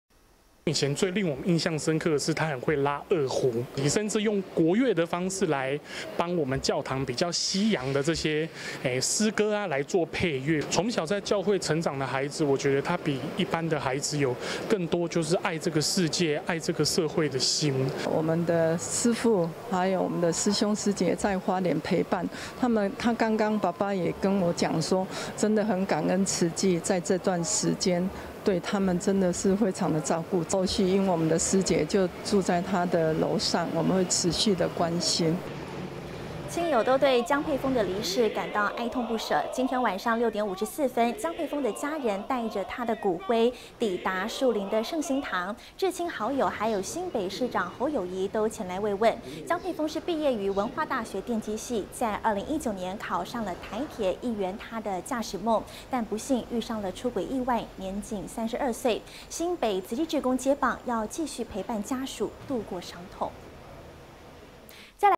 助理司機員江沛峰 告別式後骨灰送返樹林聖心堂
與袁淳修共同直接目擊強烈撞擊的還有助理司機員「江沛峰」，他的遺體，在今天上午火化，並在花蓮美崙天主教堂舉辦告別式，今天晚間將近7點鐘，家人帶著他的骨灰，返回新北樹林，落葉歸根。

親友無不哀痛不捨，今天晚上6點54分，江沛峰的家人帶著骨灰，抵達樹林的聖心堂，至親好友以及新北市長侯友宜，都前來慰問。江沛峰畢業於文化大學電機系，是在2019年考上台鐵，一圓駕駛夢。但不幸遇上出軌意外，年僅32歲。新北慈濟志工接棒，要繼續陪伴家屬，度過傷痛。
友人 黃先生：「以前最令我們印象深刻的是，(沛峰)他很會拉二胡，甚至用國樂的方式 來幫我們教堂，比較西洋的詩歌來做配樂，從小在教會成長的孩子，我覺得他比一般的孩子，有更多愛這個世界，愛這個社會的心。」

慈濟志工 莊秀里：「我們的師父 還有我們的師兄師姊，在花蓮陪伴 他爸爸剛剛也跟我講說，真的很感恩慈濟在這段時間，對他們真的是非常的照顧，後續因為我們的師姊，就住在他(家)樓上，我們會持續地關心。」

台北報導：張泰綾 李建寬。